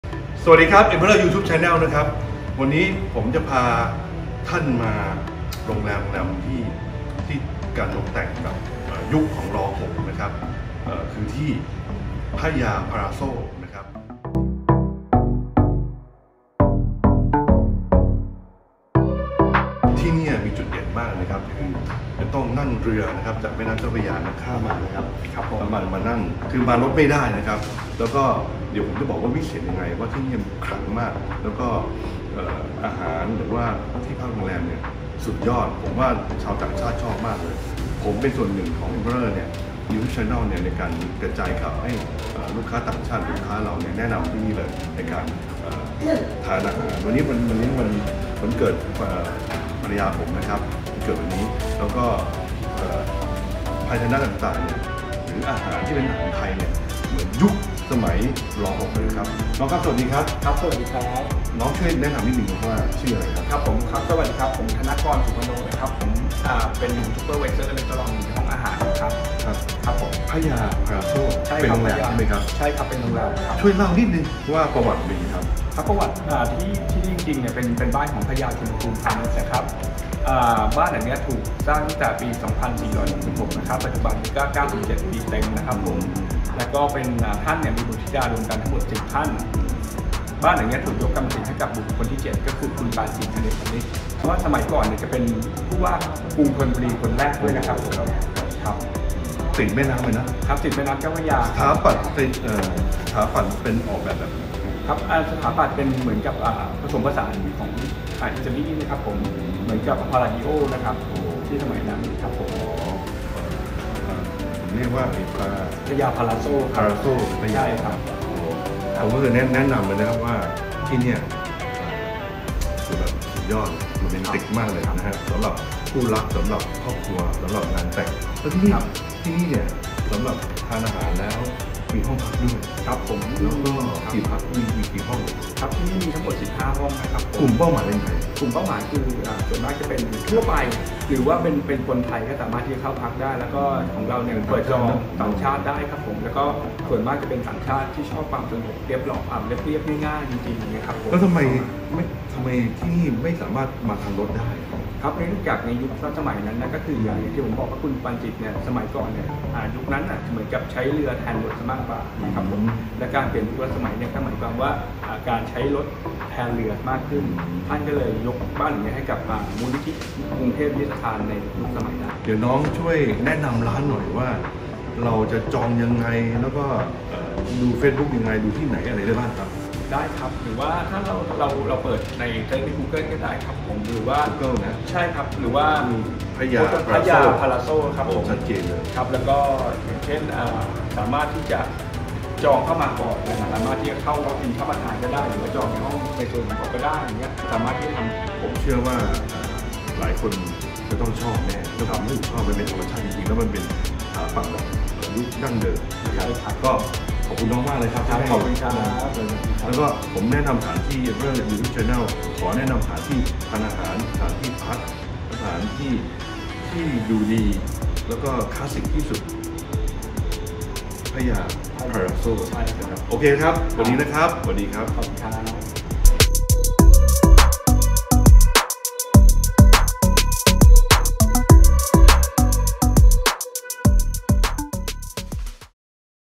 เอ็มเพอร์เรอร์ พาเที่ยว : โรงแรมเจ้าพระยา พาลาซโซ (Praya Palazzo)
พาไปชม โรงแรมสุดคลาสิกริมแม่น้ำเจ้าพระยา ที่จะพาคุณย้อนสู่ยุคสมัยรัชกาลที่ 6